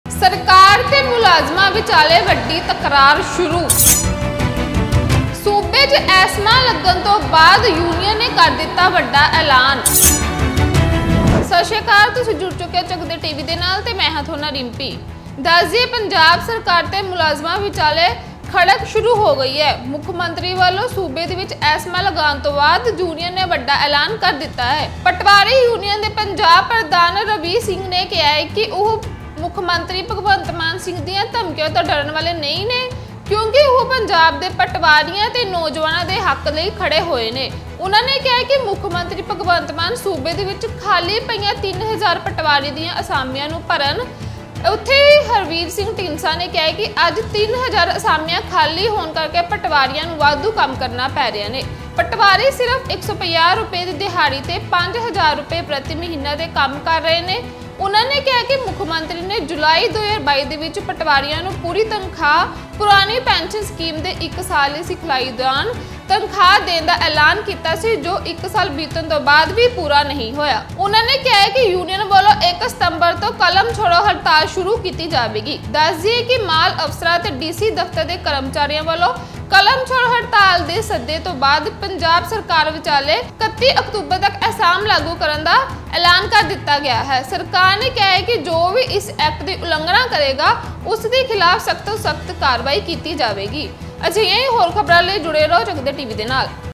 ਸਰਕਾਰ ਤੇ ਮੁਲਾਜ਼ਮਾਂ ਵਿਚਾਲੇ ਤਕਰਾਰ !...ਸੂਬੇ 'ਚ ਐਸਮਾ ਲਾਉਣ ਮਗਰੋਂ ਯੂਨੀਅਨ ਨੇ ਕੀਤਾ ਵੱਡਾ ਐਲਾਨ
ਸਰਕਾਰ ਤੇ ਮੁਲਾਜ਼ਮਾਂ ਵਿਚਾਲੇ  ਤਕਰਾਰ !
ਸੂਬੇ 'ਚ ਐਸਮਾ ਲਾਉਣ ਮਗਰੋਂ ਯੂਨੀਅਨ ਨੇ ਕੀਤਾ ਵੱਡਾ ਐਲਾਨ